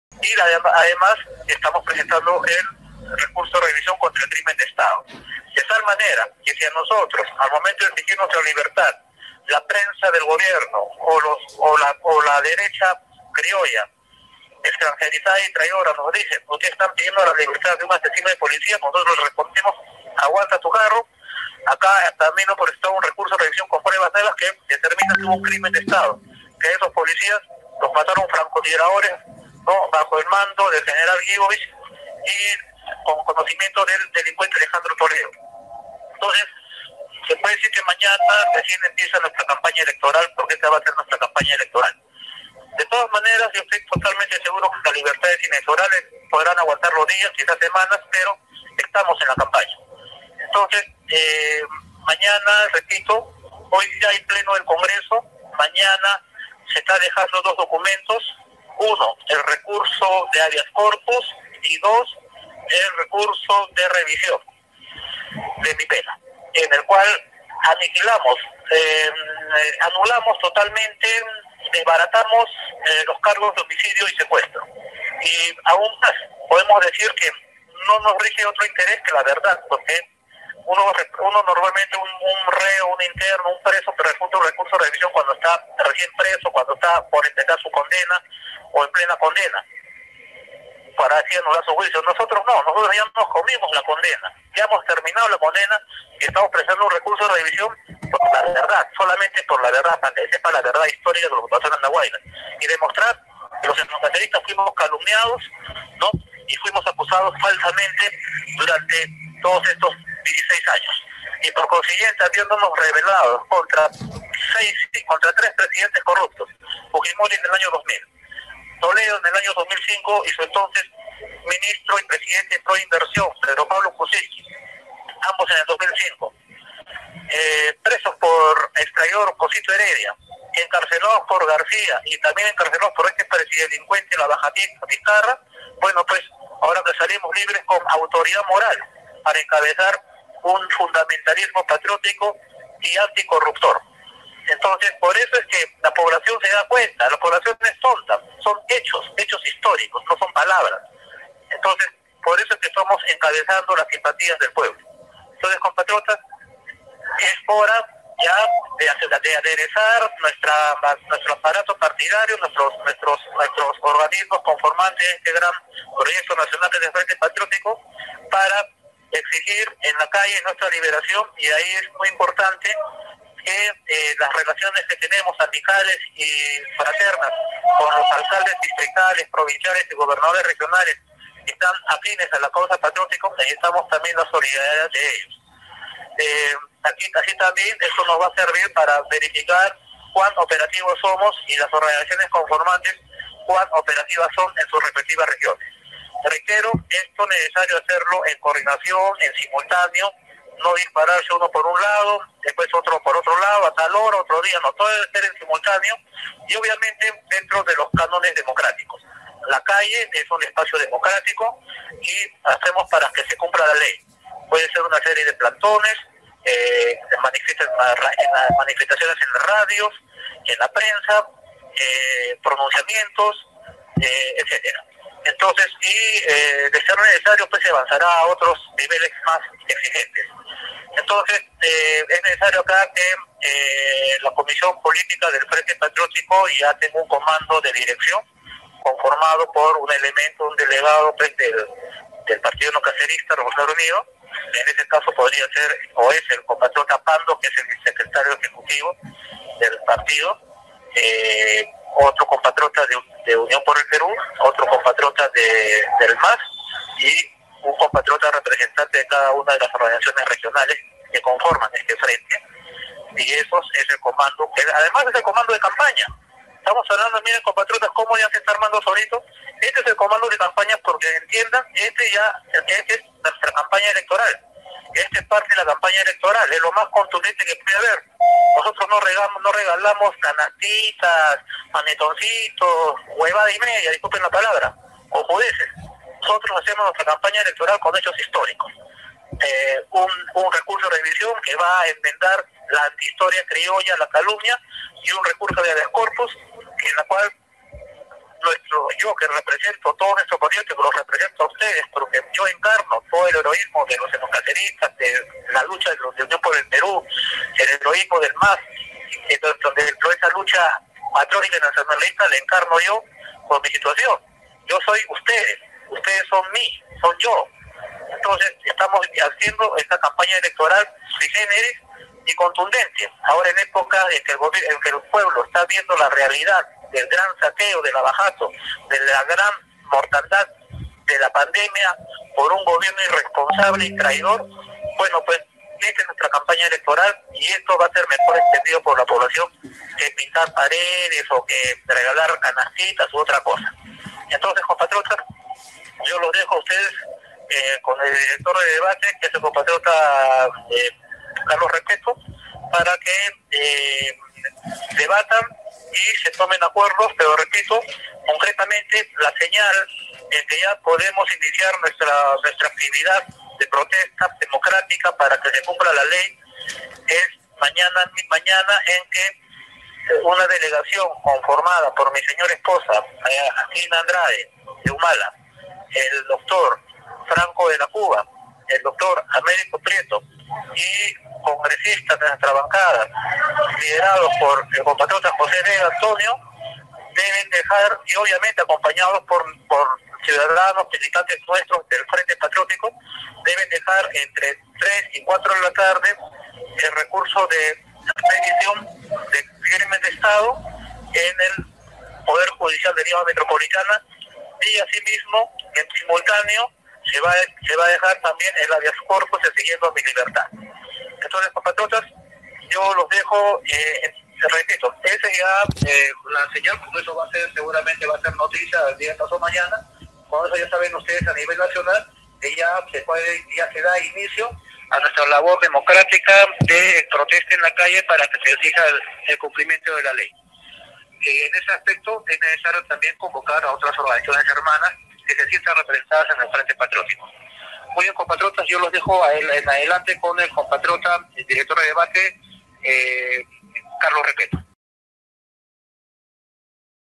Habla el My. Antauro Humala recurso de revisión.
Antauro Humala Presenta Recurso de de Revisión sobre los hechos acaecidos en Andahuaylas